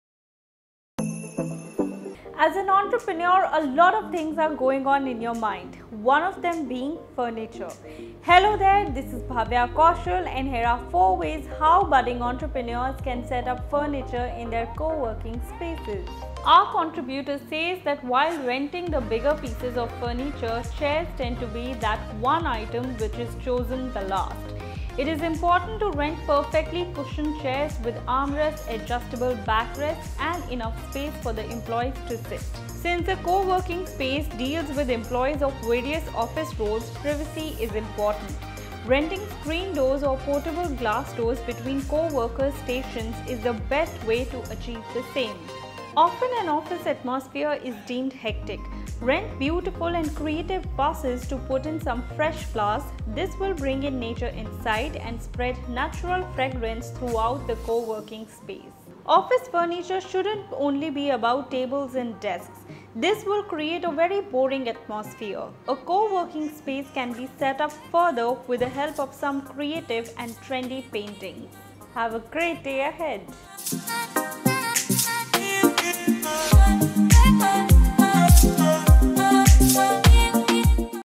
4 Things to Know For July 22: News that Entrepreneurs Can Use
Our Features Writer, Bhavya Kaushal, gives a sneak peek into the important tips for entrepreneurs. 4 Ways how Budding Entrepreneurs can Set Up Furniture In Their Co-Working Space